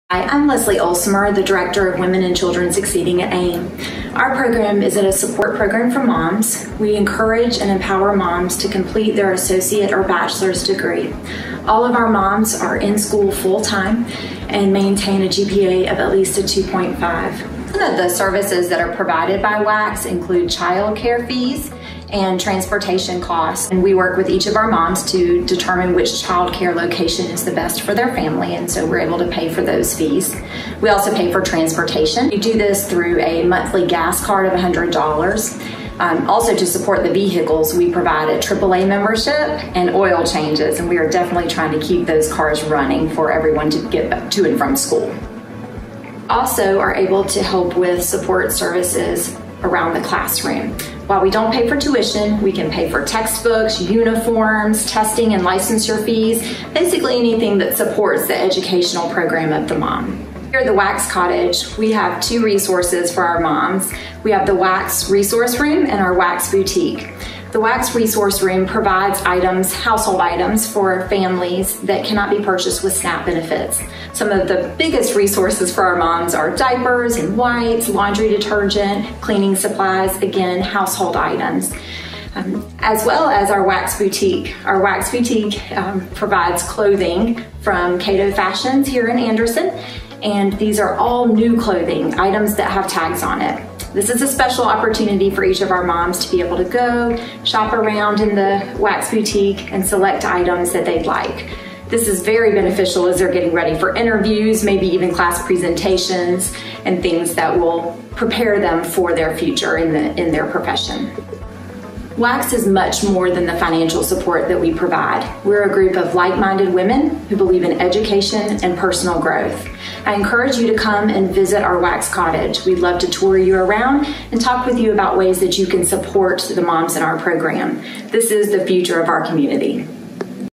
WACS (Women and Children Succeeding) What we do!
If you're a mother, thinking of going back to school, we're here to help!